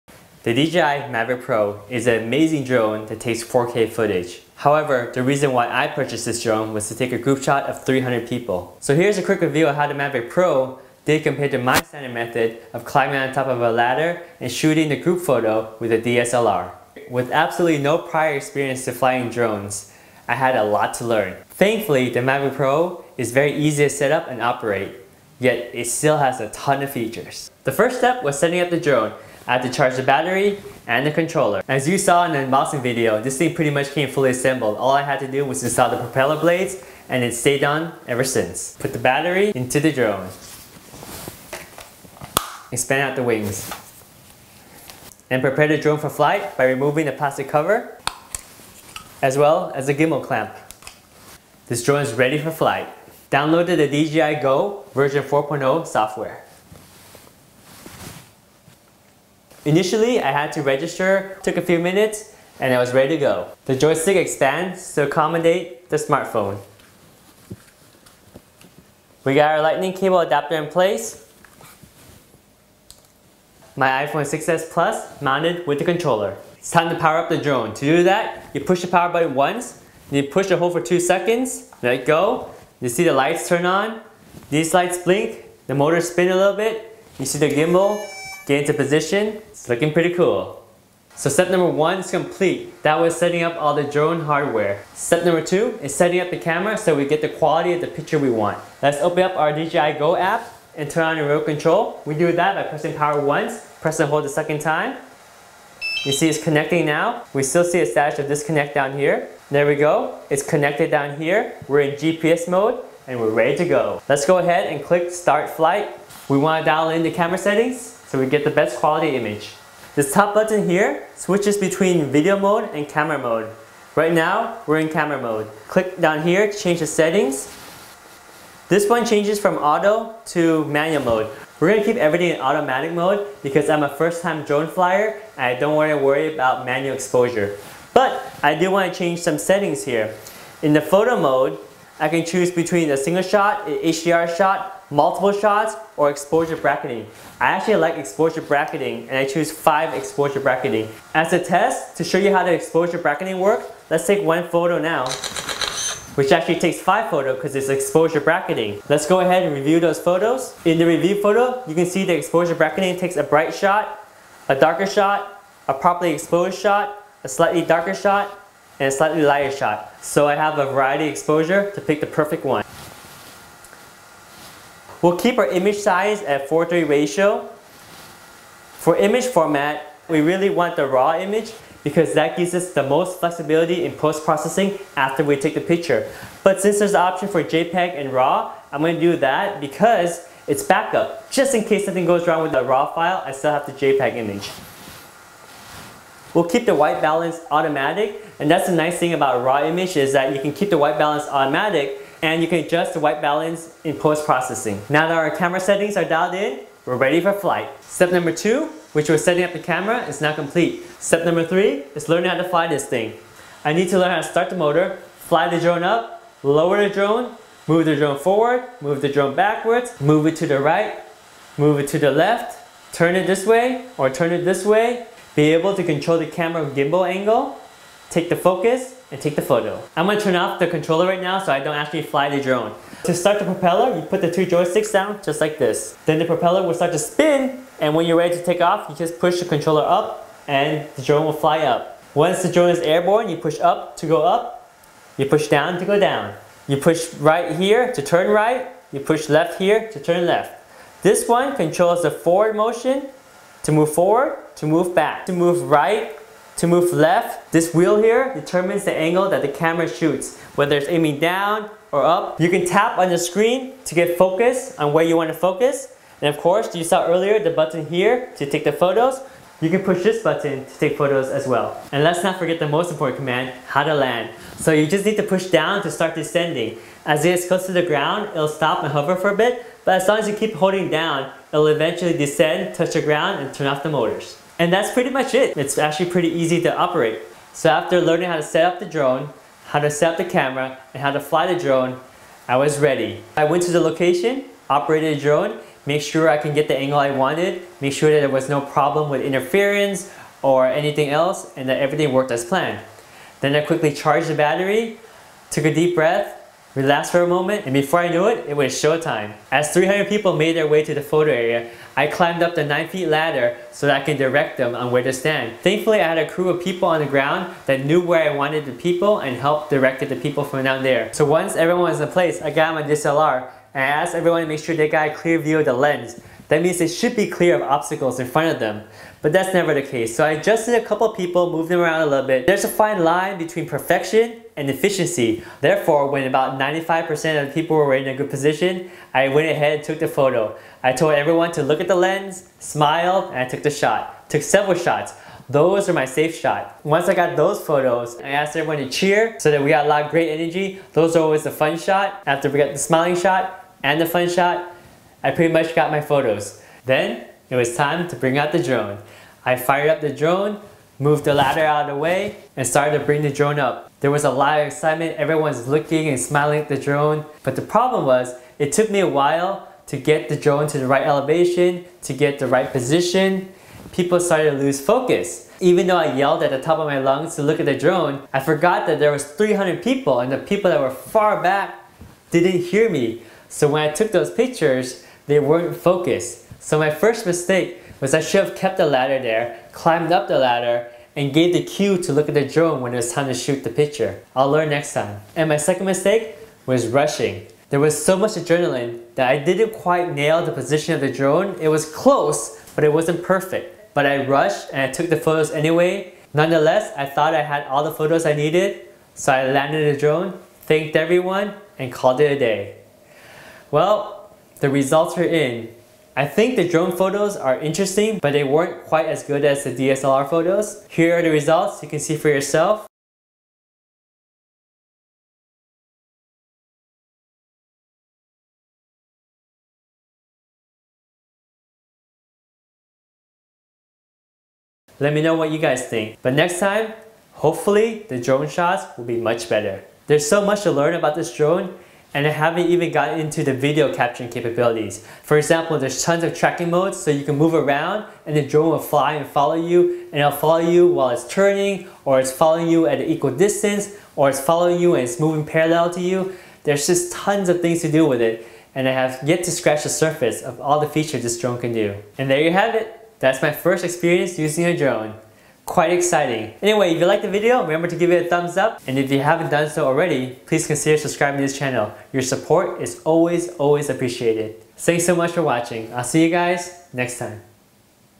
How to setup the DJI Mavic Pro Drone for large group photos (Team Photo). Compared to using a DSLR!
How to setup the DJI Mavic Pro Drone for large group photo (Team Photo). Compared to using a DSLR!

Drone (DJI Mavic Pro) vs DSLR: Large Group Photo (Team Photo)

Drone vs DSLR

DSLR: Canon 1Dx with 16-35mm f/2.8 II Lens.